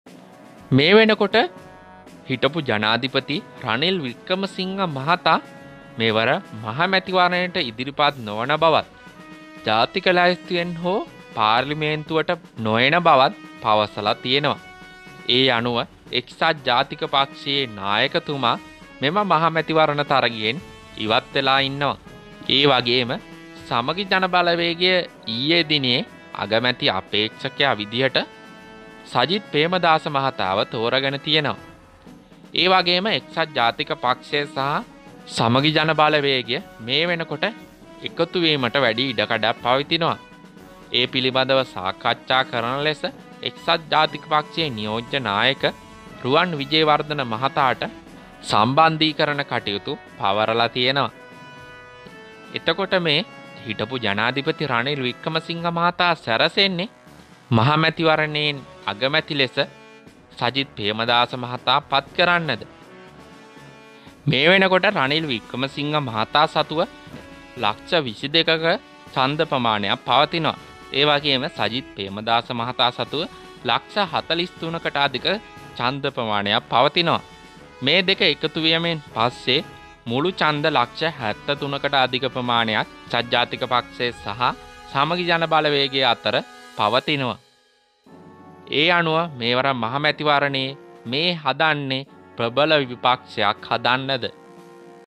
රනිල් සජිත් අගමැති කරන්නද මේ සැරසෙන්නේ❤💚👊
මෙවර 2024 මහමැතිවරණයේ එකසත් ජාතික පක්ෂයේ සහ සමගි ජන බලවේගයේ තීරණය කුමක් වේවිද?